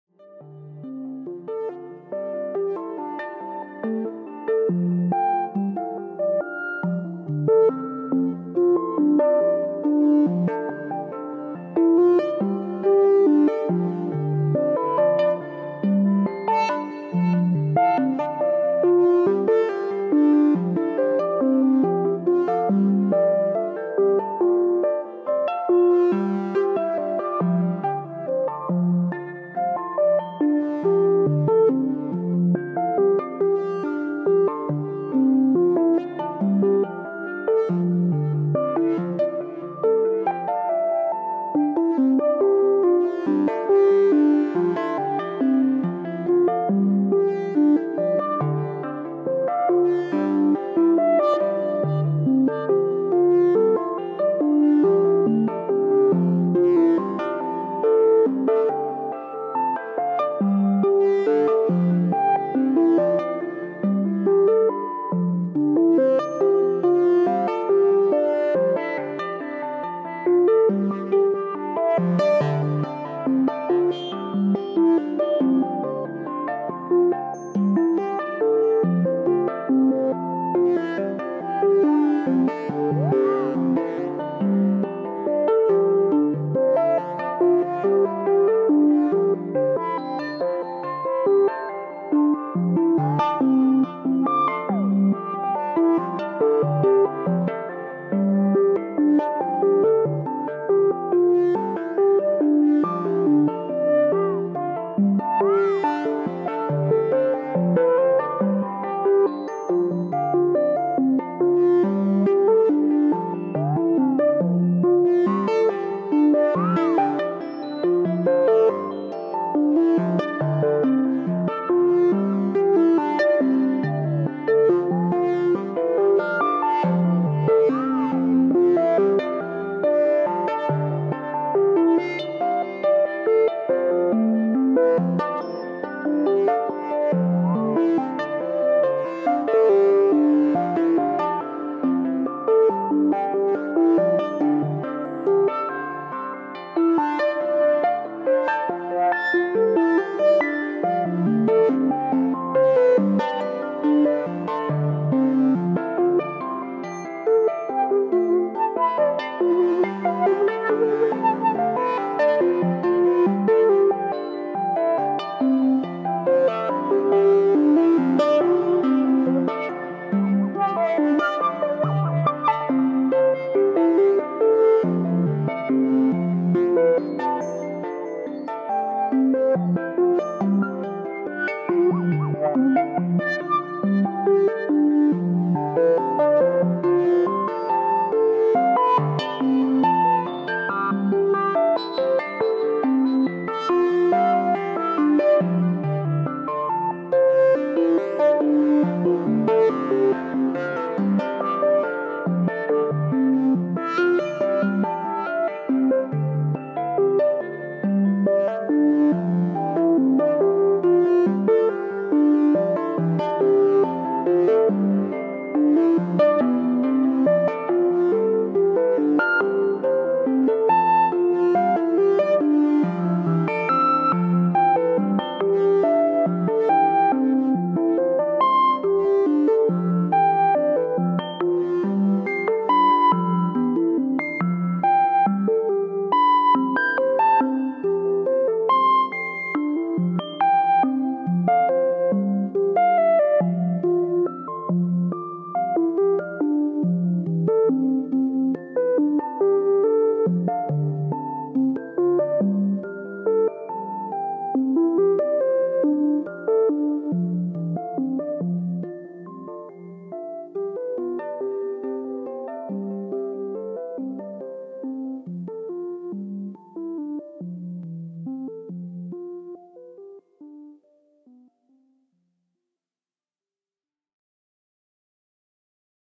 Washout
This was a fun session. I don’t have enough cables so at a certain point I just gotta get in there and wiggle stuff.

No idea what’s gonna happen when I get some more cables and modulation sources. It will either be amazing or shit.